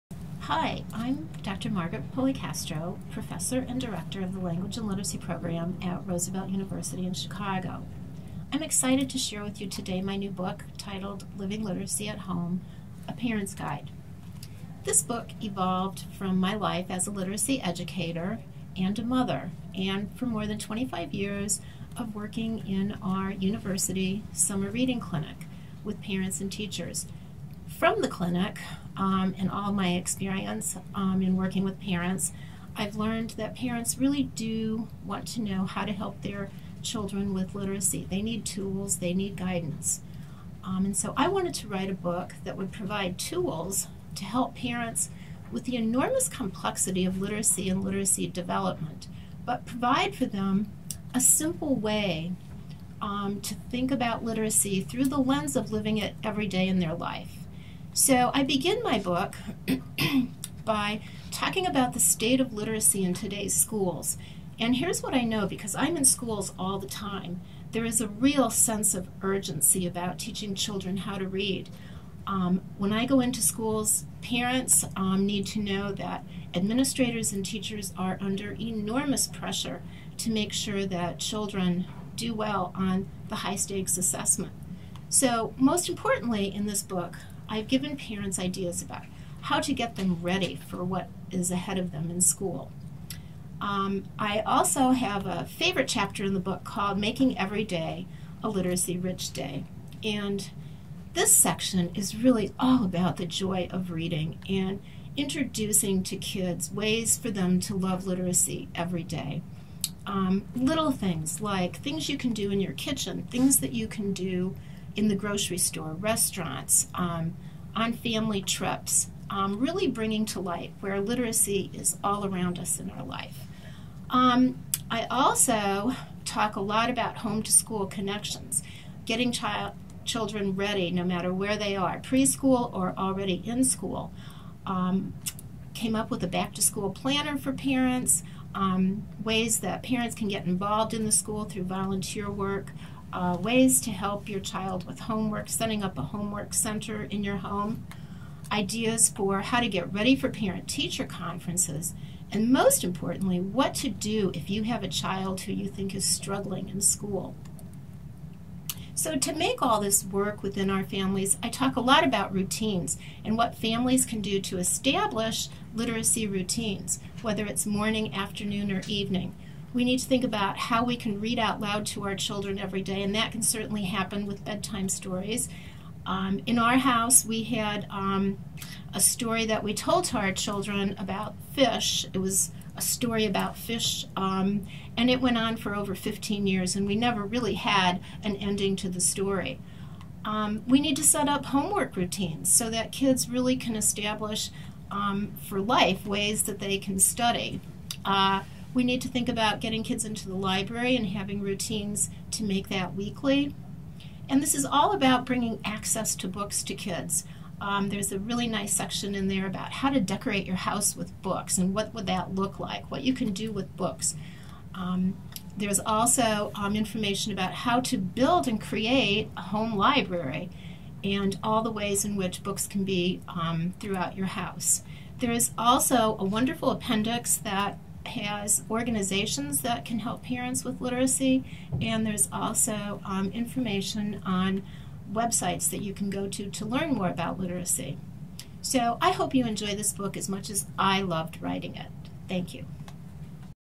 Living Literacy at Home: A Parent’s Guide
Watch Dr. Margaret Policastro, author of Living Literacy at Home: A Parent’s Guide, talk about making every day a literacy-rich day in your home. Parental involvement is crucial in helping children build language and literacy skills. Drawing on her experience as a mother of four boys, and as a literacy educator, Dr. Policastro presents straightforward, practical ideas to encourage the joy of reading with your children. Based on her 25 years of work with parents and teachers  at Roosevelt University’s Language and Literacy Summer Reading Clinic in the Chicago area, her book is the perfect resource to help you bridge classroom learning and literacy growth outside of school.